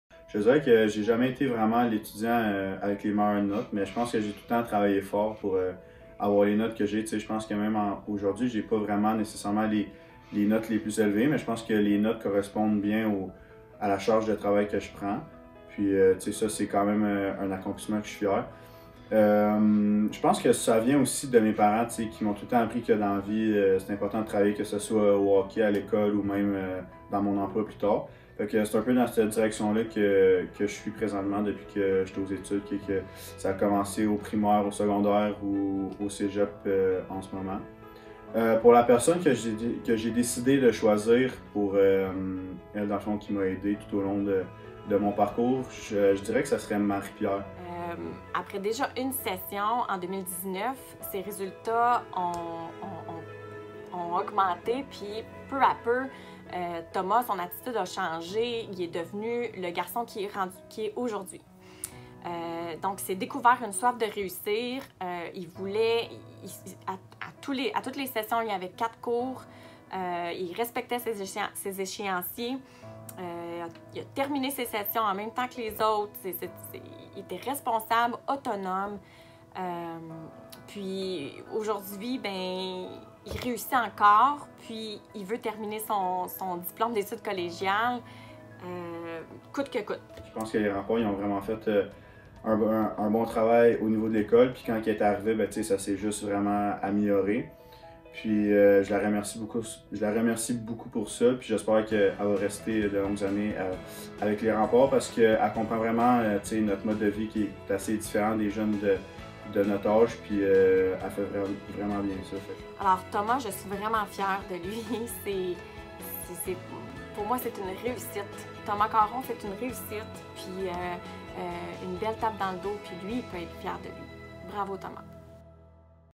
Prix Gervais-Munger 2021 | Thomas Caron, Québec
Faire preuve de persévérance, c’est de ne pas avoir peur de s’engager, de foncer et de se montrer patient pour atteindre un ou des buts tant au niveau personnel, scolaire ou professionnel.  C’est un trait de caractère fondamental pour relever tout défi auquel une personne peut faire face dans la réalisation d’un projet, d’un rêve. Dès ses débuts dans le domaine compétitif du hockey, un joueur doit rapidement apprendre à forger cet aspect afin de poursuivre ses rêves. Être persévérant devient alors une force, un atout qui l’aidera assurément à s’épanouir non seulement au niveau du hockey, mais également au niveau scolaire.